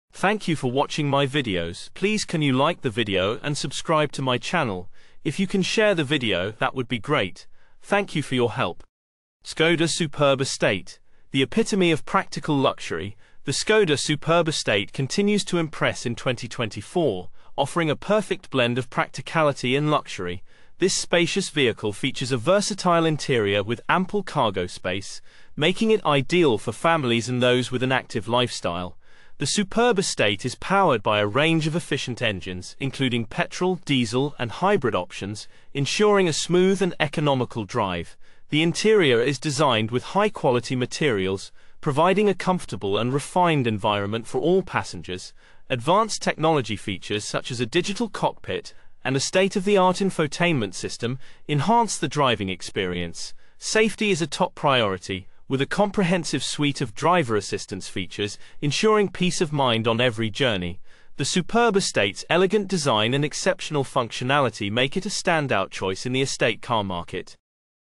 Skoda Superb Estate: The Epitome of Practical Luxury
The Skoda Superb Estate continues to impress in 2024, offering a perfect blend of practicality and luxury. This spacious vehicle features a versatile interior with ample cargo space, making it ideal for families and those with an active lifestyle. The Superb Estate is powered by a range of efficient engines, including petrol, diesel, and hybrid options, ensuring a smooth and economical drive. The interior is designed with high-quality materials, providing a comfortable and refined environment for all passengers. Advanced technology features, such as a digital cockpit and a state-of-the-art infotainment system, enhance the driving experience. Safety is a top priority, with a comprehensive suite of driver assistance features ensuring peace of mind on every journey. The Superb Estate's elegant design and exceptional functionality make it a standout choice in the estate car market.